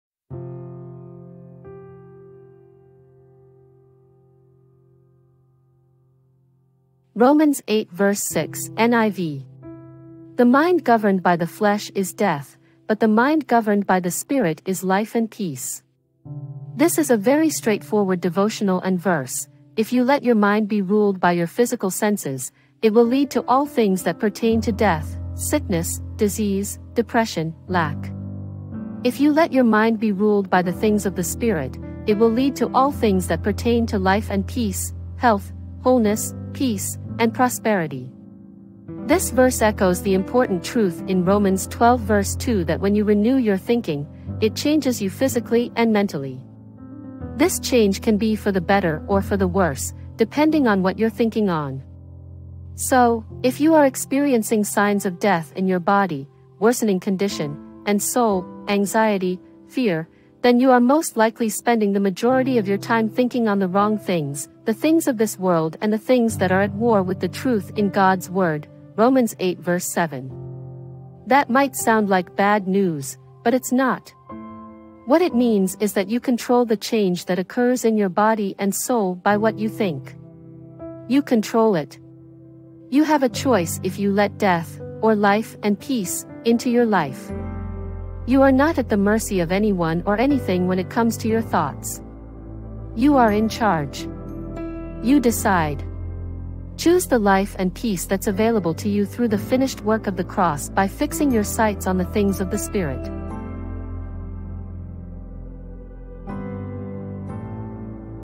The Mind Set on the Spirit, Not the Flesh (Romans 8:6)
Romans 8:6 says, 'The mind governed by the flesh is death, but the mind governed by the Spirit is life and peace.' This verse contrasts having our minds set on earthly things versus having our minds set on spiritual things. A fleshly mindset leads to death and brokenness, while a spiritual mindset leads to life and peace.

This video explores the contrast Paul draws in Romans 8:6. The 'flesh' refers to an outlook focused on worldly priorities and self-effort. The 'Spirit' refers to an outlook aligned with God's will and empowered by Him. The video encourages viewers to adopt a spiritual mindset, surrendered to God and dependent on Him. Only by embracing a Gospel-shaped perspective, not a flesh-driven one, can we experience the life and peace God intends for us. Our thoughts and values must be transformed by the renewing of our minds according to the Spirit, not the flesh.